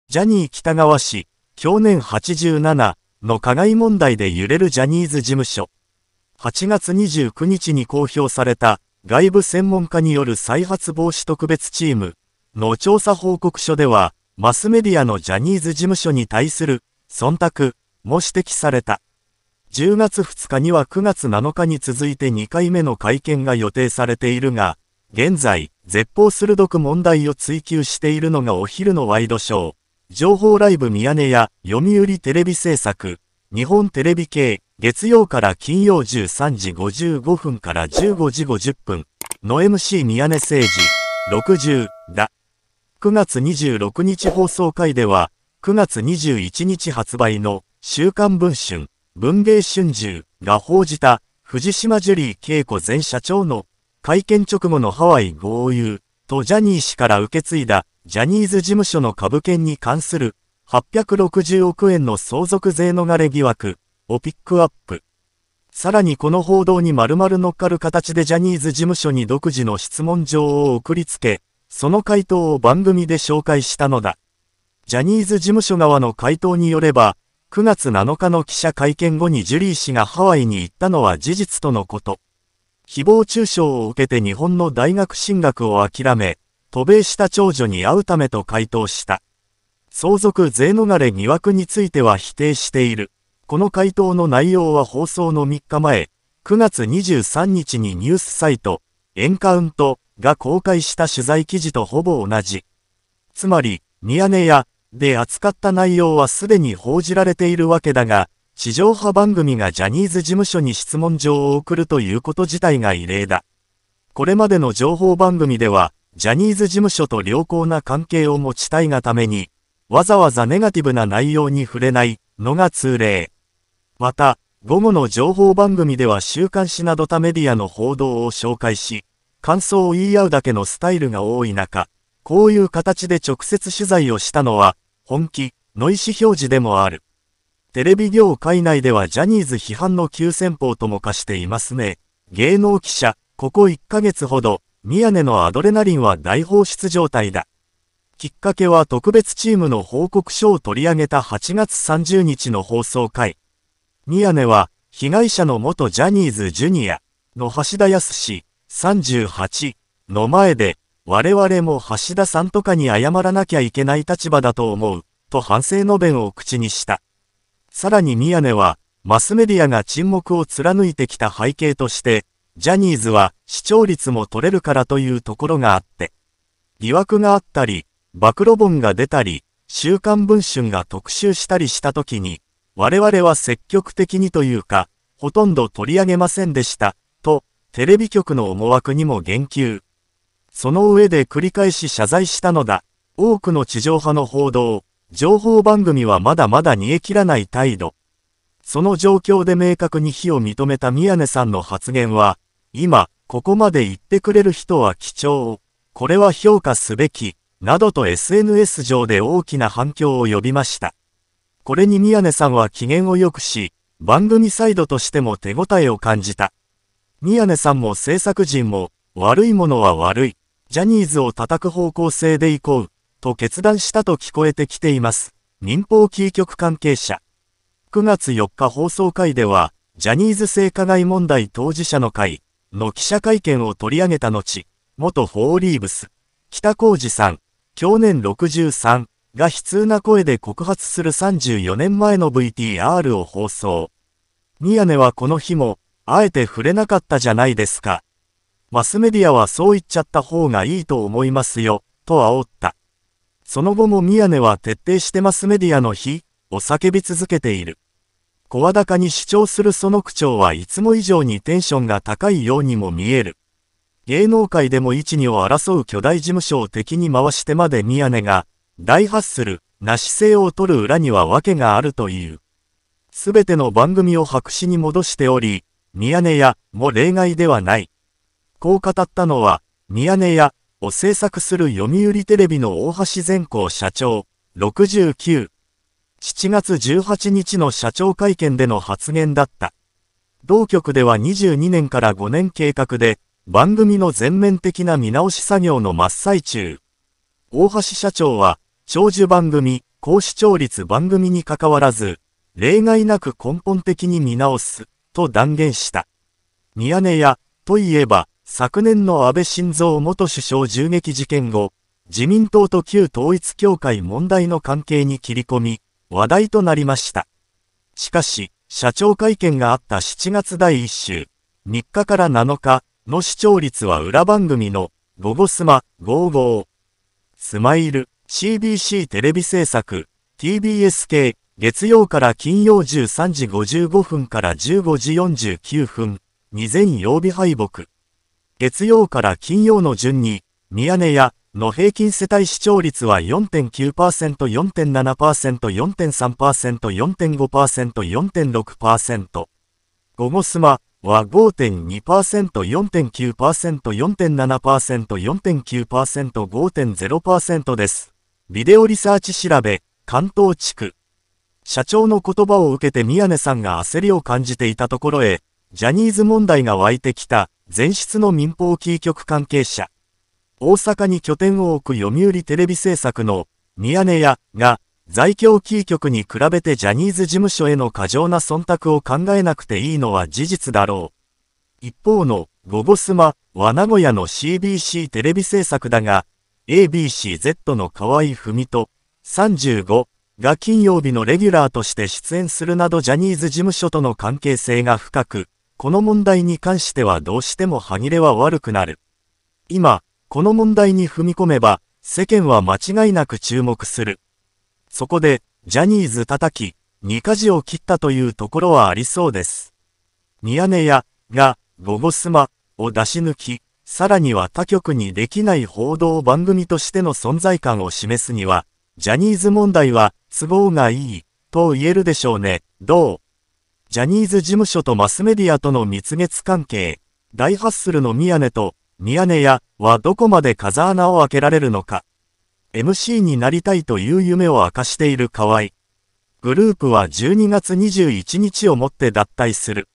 宮根誠司が大ハッスル!!『ミヤネ屋』異例の週刊文春に丸乗っかりのジャニーズ叩き！河合郁人出演の『ゴゴスマ』出し抜き戦略!?(‎@Tokyorends )
宮根誠司が大ハッスル!!『ミヤネ屋』異例の週刊文春に丸乗っかりのジャニーズ叩き！河合郁人出演の『ゴゴスマ』出し抜き戦略!?(@Venom60k )